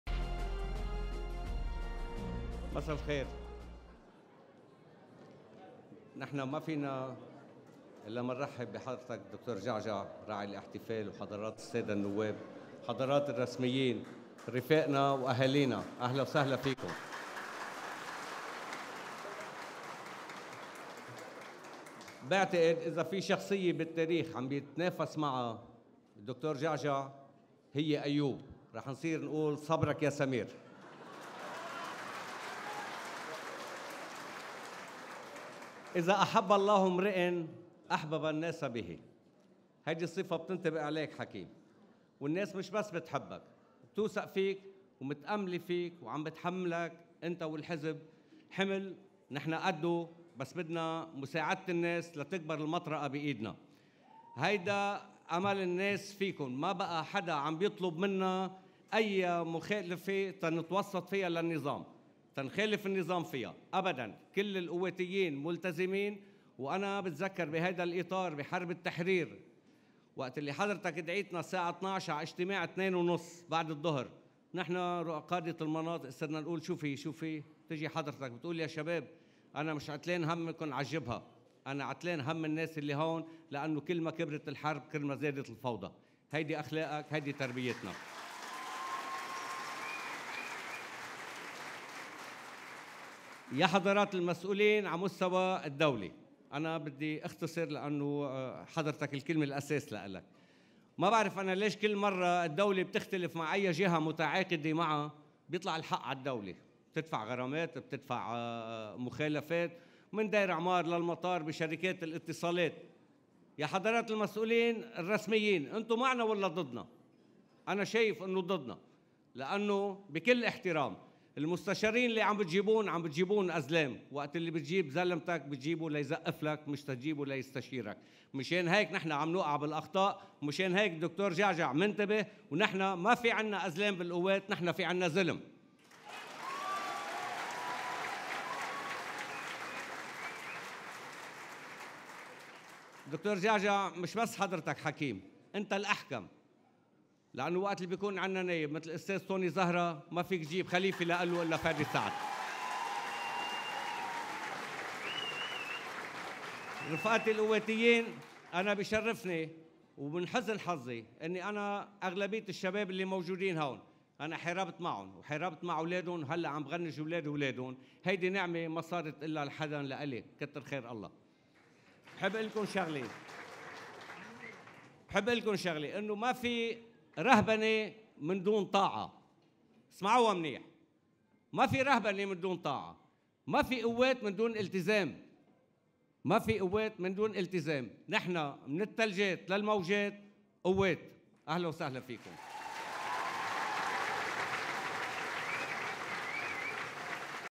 كلمة منسق “القوات اللبنانية” عصام الخوري خلال مهرجان إنتخابي نظمته “القوات” – البترون 27-4-2018
أكد منسق “القوات اللبنانية” في قضاء البترون عصام الخوري أنه إذا أحب الله إمرءا احبب الناس به. وتوجه الى جعجع بالقول :”حكيم، ليس الناس وحدهم يحبونك كذلك يثقون بك ويأملون أن ا”لقوات” ستبقى واقفة في وجه الفساد والصفقات”، متسائلا: “لماذا كلما الدولة اختلفت مع جهة متعاقدة معها يكون الحق على الدولة وتدفع غرامات وتعويضات من دير عمار للمطار ….”
ثم توجه الى المسؤولين قائلا:” انتم مع من ؟معنا أو ضدنا؟ مجيبا: “أكيد ضدنا لأن أزلامكم مستشارين.
وتوجه الى الدكتور جعجع: “لست حكيما بل أنت الاكثر حكمة لأنه عندما يكون عندك نائب مثل أنطوان زهرا لا تستطيع ان تختار إلا فادي سعد ليملأ مكانه ويضيف عملا على ما قام به. قد تكون هذه المرة الاخيرة وقد تكون مرة اخرى بـ ۷ ايار الانتصار ليس لنتبادل التهاني ونهنىء “القوات” لكن لكي نهنىء الناس بإختيارهم لفادي سعد.”
وتوجه الى رفاقه: “القواتيين وأهلنا الطيببين هلق لا يجوز ان يذهب اي صوت للترف الانتخابي. كل صوت له تأثير لا تتركوا صوتكم يذهب سدى .البترون ليست قضية وإيمان، البترون قضية ايمان بالله و”القوات” ولبنان.”
وختم:” لا رهبنة من دون طاعة ولا قوات من دون التزام ونحنا من التلجات للموجات قوات.”